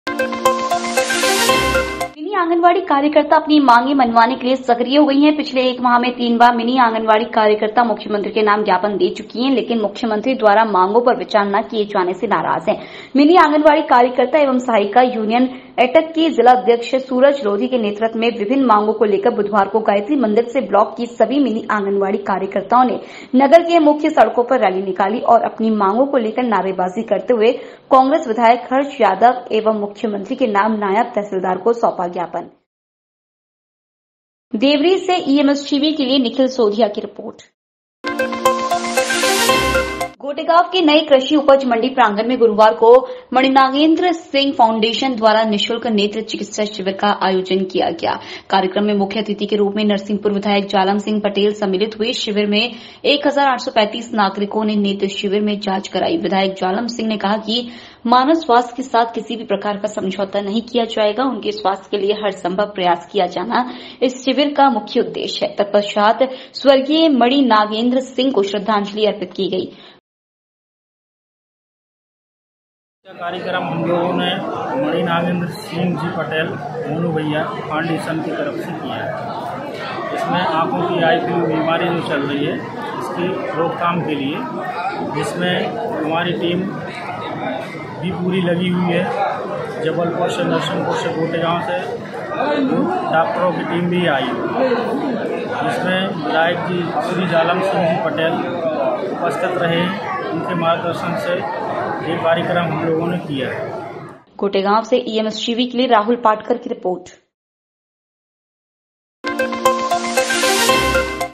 मणिनागेंद्र सिंह फाउंडेशन द्वारा आई फ्लू नि.शुल्क नेत्र शिविर आयोजित| EMS TV 10-Aug-2023
गोटेगांव के नई कृषि उपज मंडी प्रांगण में गुरूवार को मणिनागेंद्र सिंह फाउंडेशन द्वारा नि.शुल्क नेत्र चिकित्सा शिविर का आयोजन किया गया कार्यक्रम में मुख्य अतिथि के रूप में नरसिंहपुर विधायक जालम सिंह पटेल सम्मिलित हुए शिविर मे 1835 नागरिको ने नेत्र शिवर मे जाच कराई विधायक जालम सिंह ने कहा कि मानव स्वास्थ्य के साथ किसी भी प्रकार का समझौता नहीं किया जाएगा उनके स्वास्थ्य के लिए हर संभव प्रयास किया जाना इस शिविर का मुख्य उद्देश्य है तत्पश्चात स्वर्गीय मणिनागेंद्र सिंह को श्रद्धांजलि अर्पित की